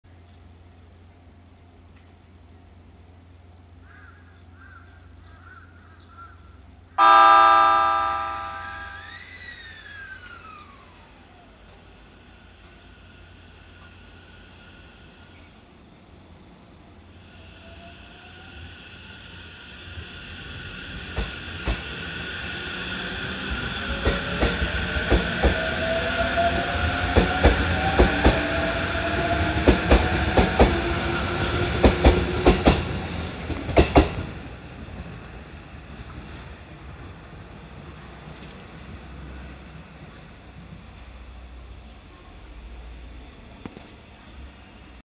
【川越線に高崎線特急のE257系？！】E257系5500番台OM-54編成 川越車輪転削返却回送警笛を鳴らして日進駅発車
回8626M 南古谷-大宮
回8927M 大宮-東大宮操